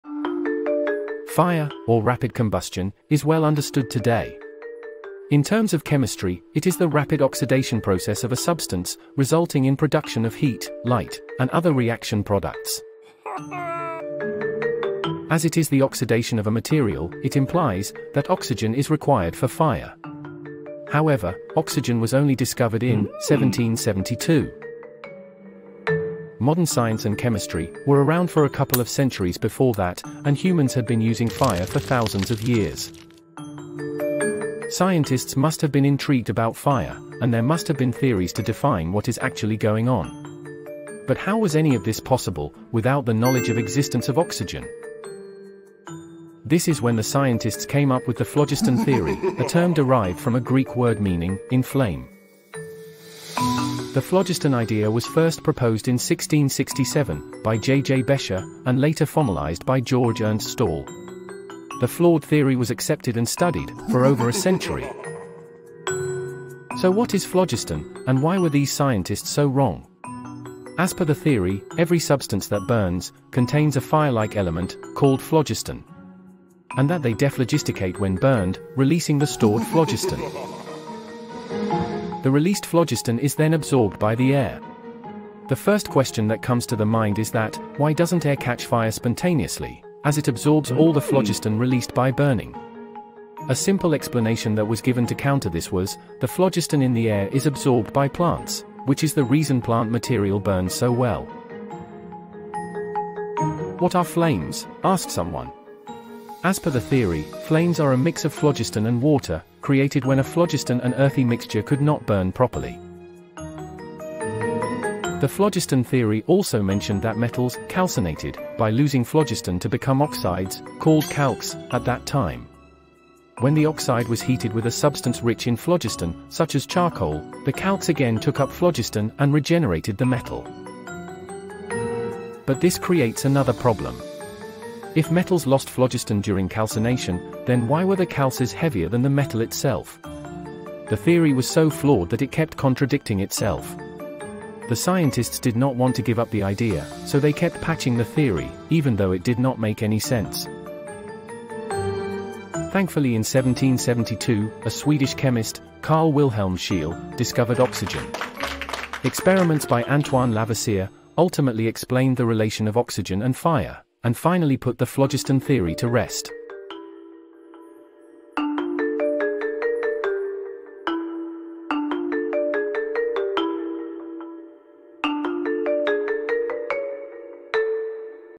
The Flaming Lie: How Scientists Were Fooled for Over a Century
Scientists were totally wrong about fire for more than one hundred years.

In terms of chemistry, Fire is the rapid oxidation process of a substance, resulting in production of heat, light and other reaction products.

However, oxygen was only discovered in 1772. Before the discovery of oxygen,  the scientists came up with the Phlogiston theory, a term derived from a Greek word meaning "inflame".

The Phlogiston idea was first proposed in 1667 by JJ Becher and later formalized by Georg Ernst Stahl.

The phlogiston theory also mentioned that metals "calcinated", by losing phlogiston to become oxides, called calx, at that time. When the oxide was heated with a substance rich in phlogiston, such as charcoal, the calx again took up phlogiston and regenerated the metal.

In 1772 a Swedish chemist, Carl Wilhelm Scheele, discovered oxygen. Experiments by Antoine Lavoisier ultimately explained the relation of oxygen and fire.